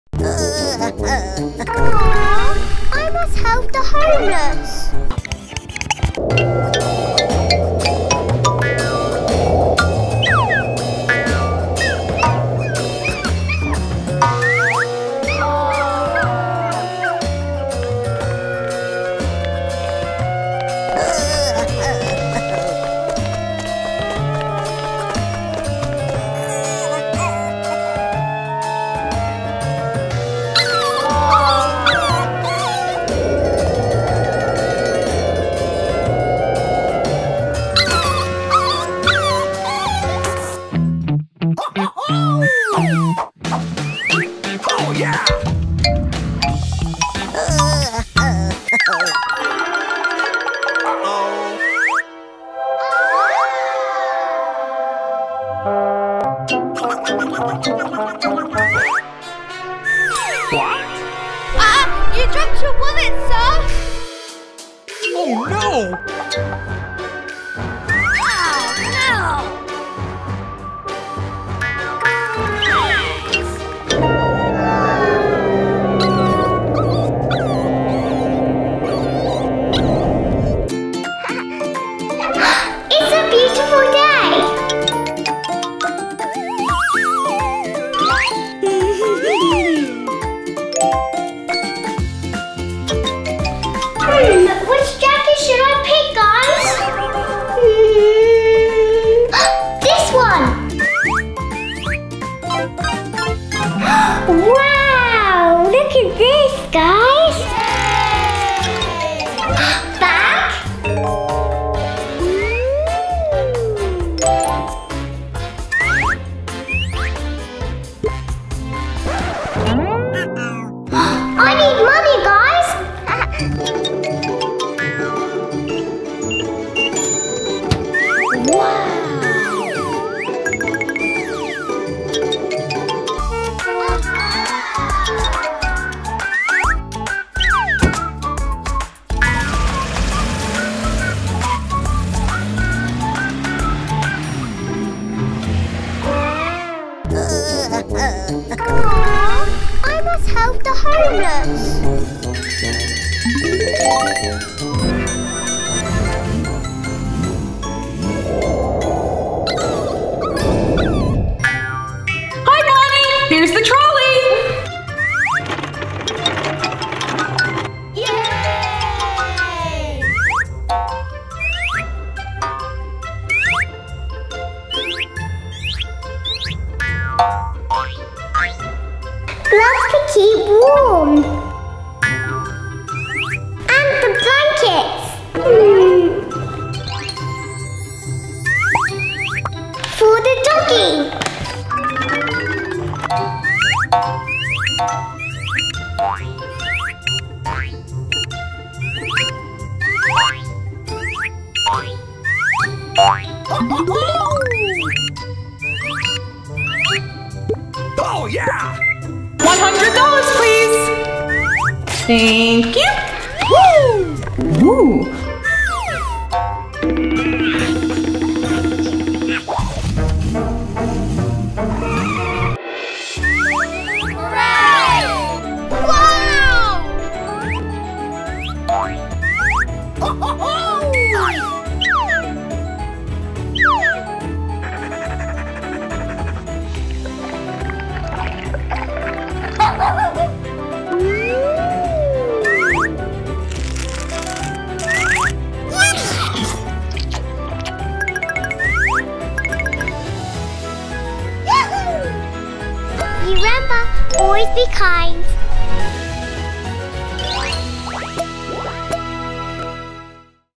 Ruby and Bonnie helps homeless person and learns to be kind for kids
Ruby and Bonnie pretends to be a good citizen as they help a homeless person by buying food groceries from the supermarket in this funny story about kindness and helping others.